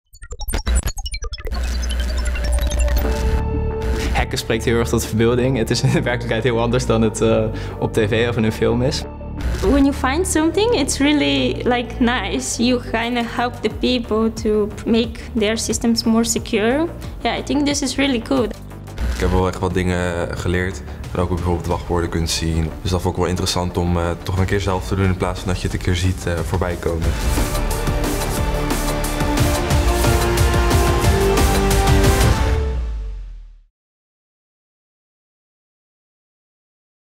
Doe mee met het hackevent voor onderwijs (HALON)!
Ben jij een student en wil jij jouw talent ontwikkelen als ethisch hacker? Doe dan mee met HALON  (Hack AL het Onderwijs Nederland). Dit is een hackevenement voor het onderwijs door het onderwijs.

Tijdens dit event mag je met je team op zoek naar kwetsbaarheden bij verschillende onderwijsinstellingen. Door kwetsbaarheden te vinden, draag je ook bij aan de cyberweerbaarheid van deze instellingen. De organisatie is in handen van SURF, het ict-samenwerkingsverband voor onderwijs en onderzoek.

Meer weten? Lees meer over HALON